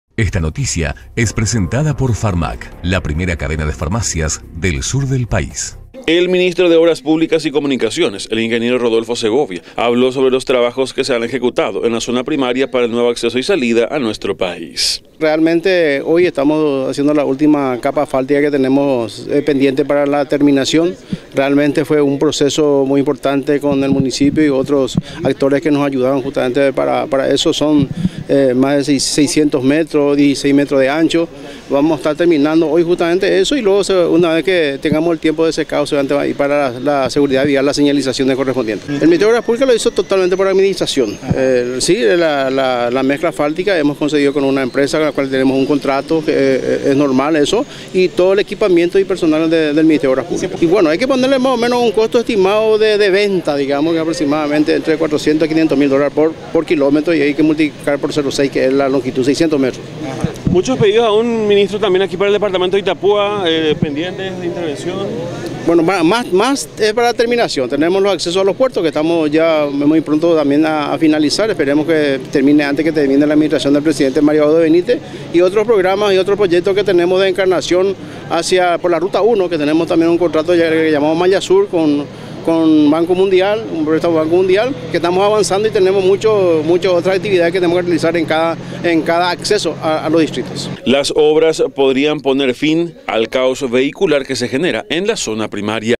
Ministro de Obras resalta finalización de la capa asfáltica para el nuevo acceso al puente San Roque
El Ministro de Obras Públicas y Comunicaciones, el Ingeniero Rodolfo Segovia, ofreció detalles sobre los trabajos que se han ejecutado en la zona primaria para el nuevo acceso y salida del país.

Según el encargado de la secretaría, la capa asfáltica ya ha sido concluida en una extensión de aproximadamente 800 metros por 16 metros de ancho. Este avance es de gran importancia para lograr descongestionar el paso fronterizo entre Encarnación y Posadas, y se espera que traiga consigo una solución a esta situación.

La finalización de la capa asfáltica en la zona primaria para el nuevo acceso y salida del país es un avance significativo en la construcción de esta importante obra. Se espera que esto contribuya a descongestionar el paso fronterizo entre Encarnación y Posadas, mejorando así la movilidad y el comercio entre ambos países.